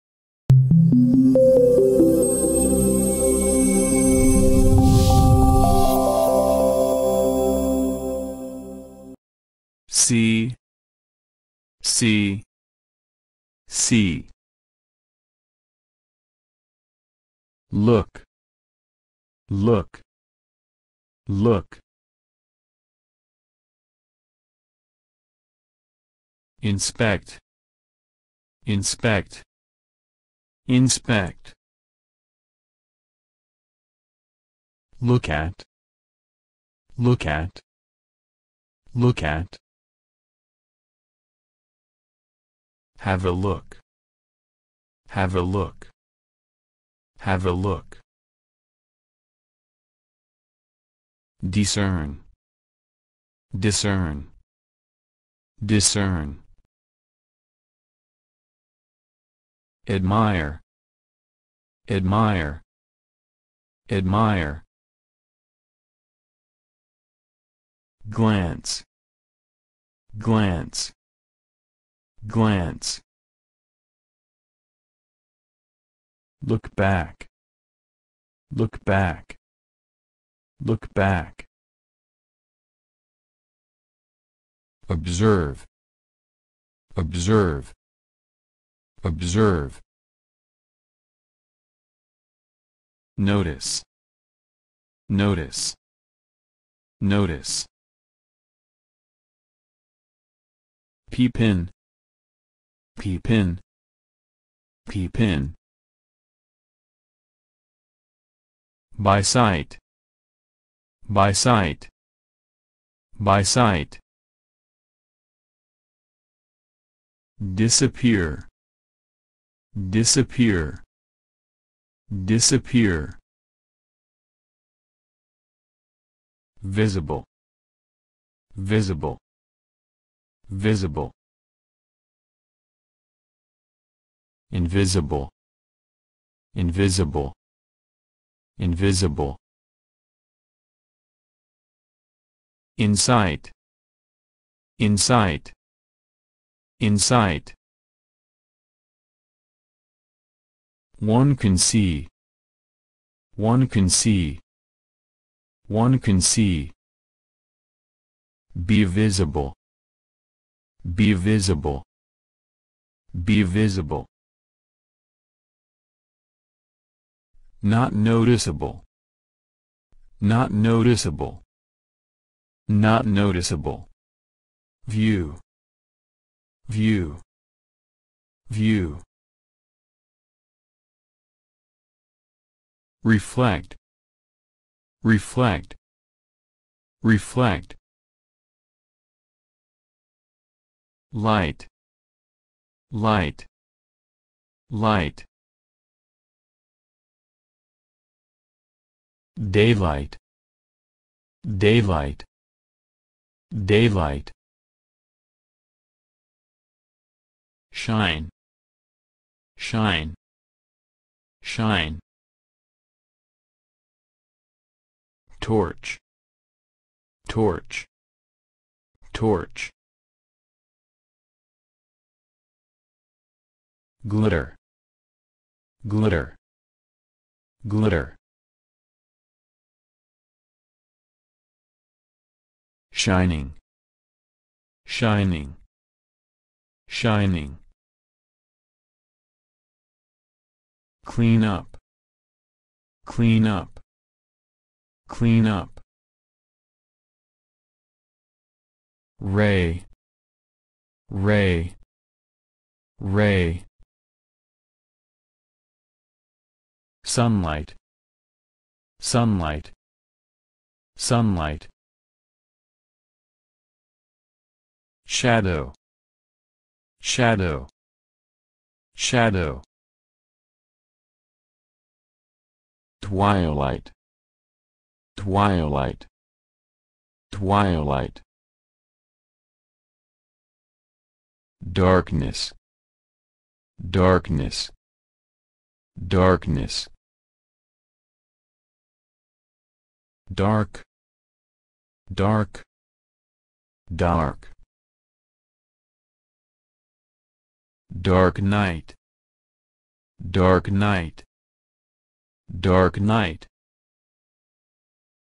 Тема: "Зрение" - Словарный запас школы GeniusEnglish
Скачать бесплатно карточки для изучения английских слов

в этом видео вы узнаете глаголы на тему зрения .А так же фразы на тему видения.